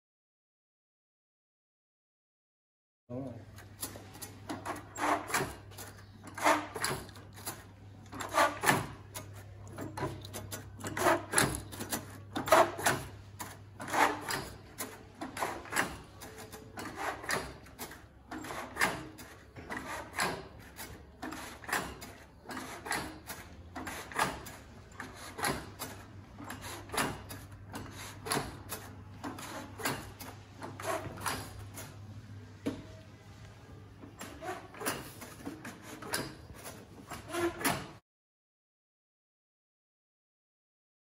Growcoon hand dispenser in gebruik bij Johan Nouws stekbedrijf.
Om de Growcoon te verwerken is er een hand dispenser ontwikkeld. Voor meer info doe navraag  bij Klasmann Deilmann Benelux